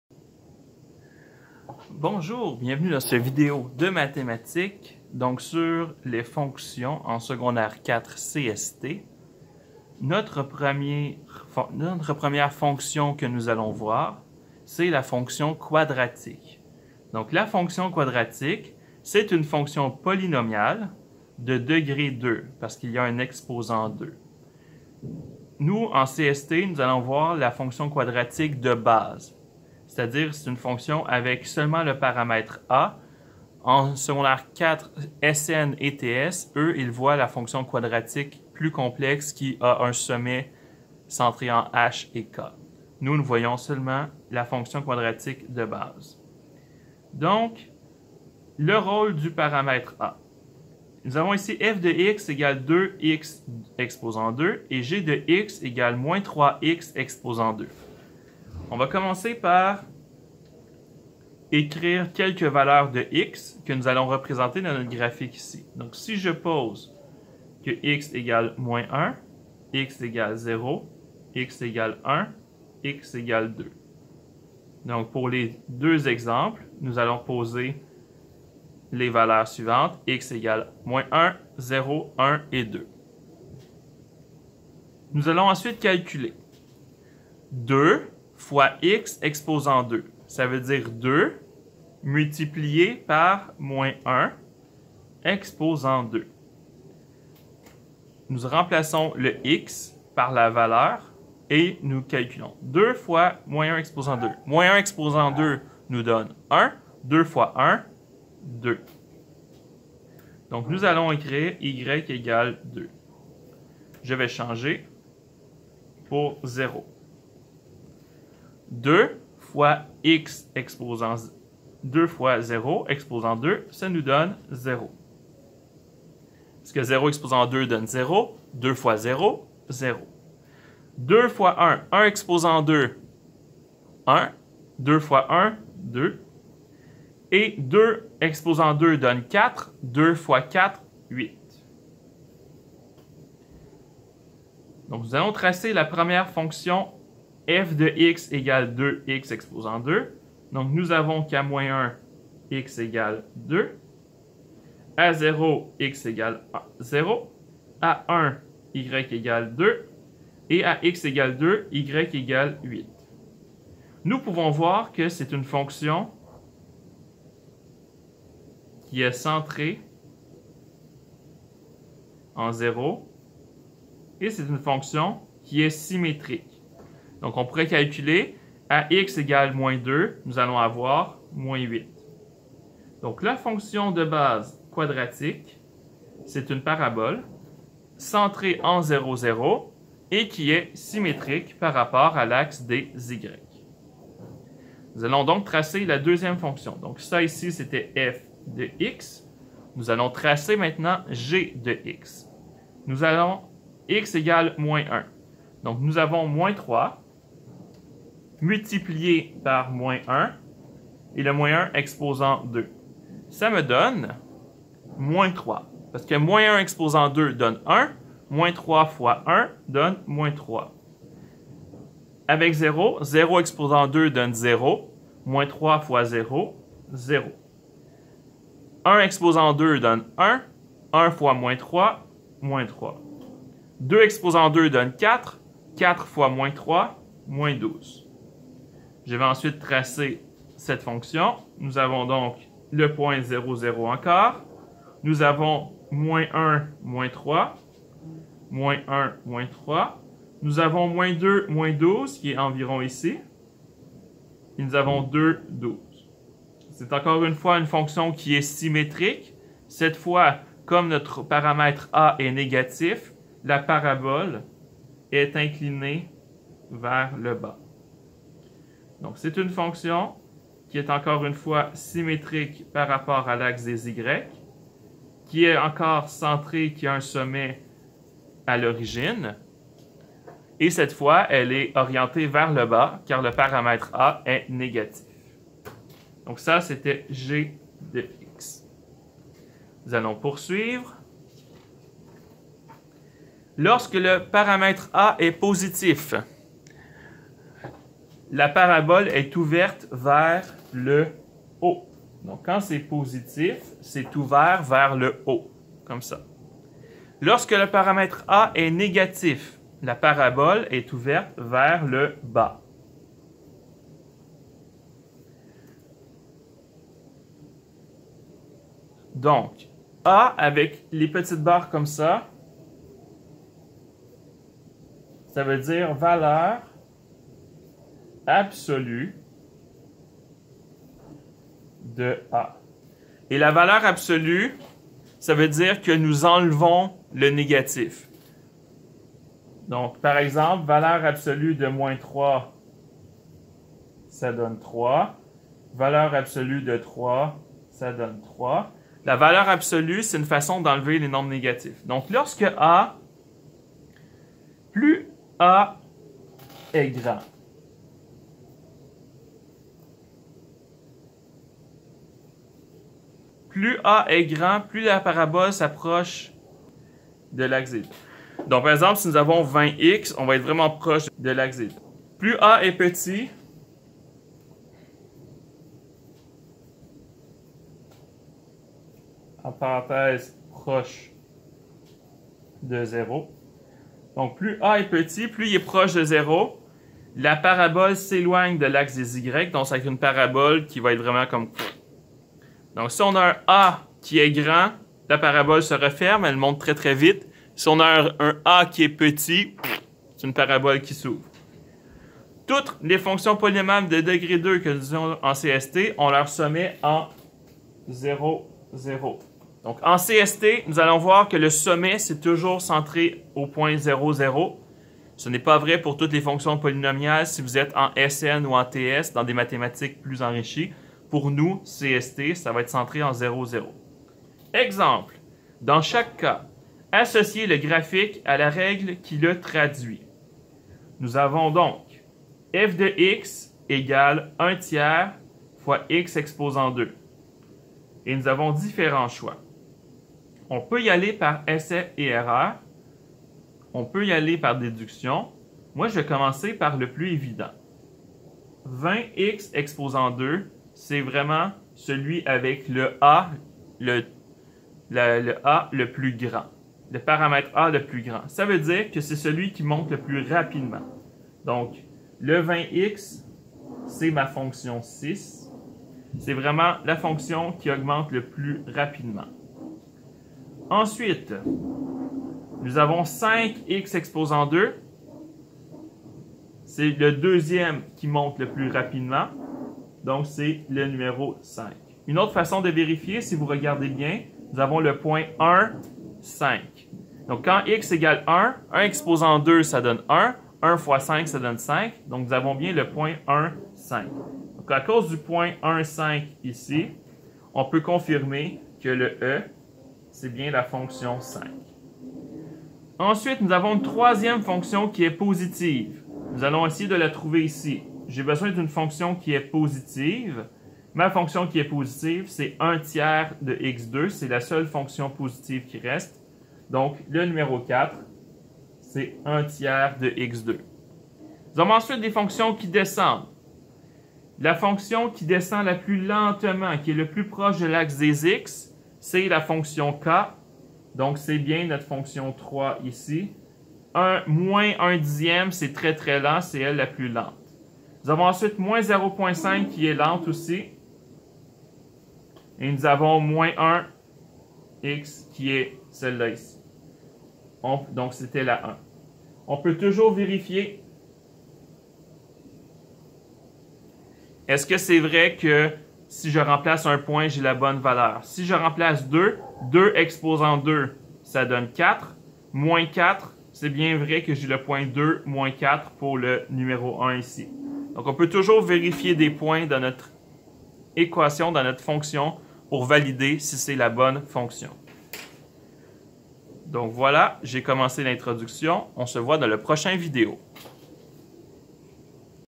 2.1A Graphique d'une fonction quadratique et paramètre a
Tracer le graphique d'une fonction quadratique en CST4
Effet du paramètre a